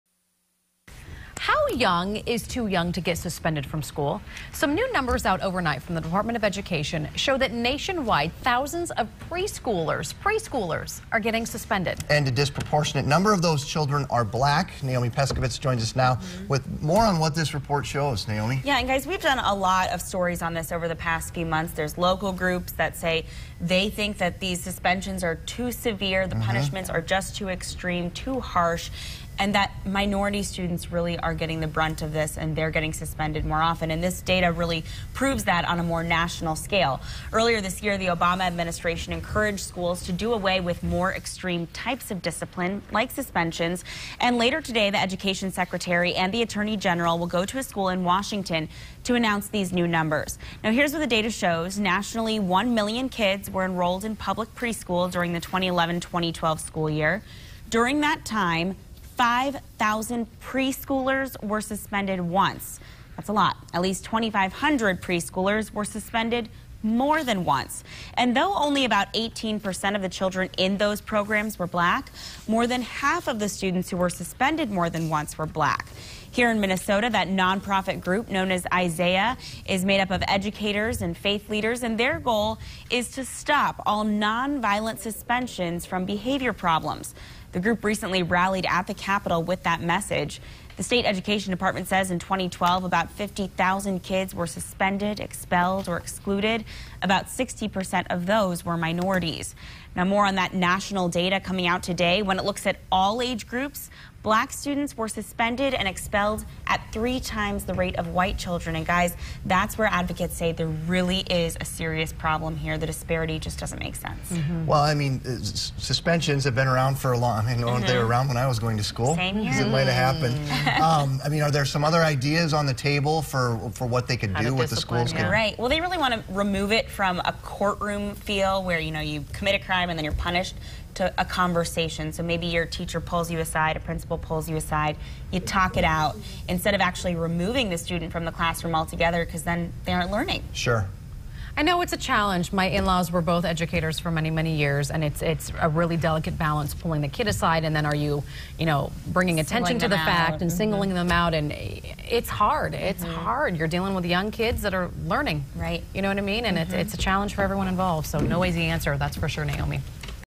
Study: Black Preschoolers More Likely To Face Suspension Than Whites
Today, the Education Department's civil rights arm released a report highlighting the racial disparities in American education.

According to the research, black students of all ages are expelled at a rate three times higher than that of white children, starting at the earliest stages of education.

The new report follows the Obama administration's release in January of new guidelines governing school discipline policies.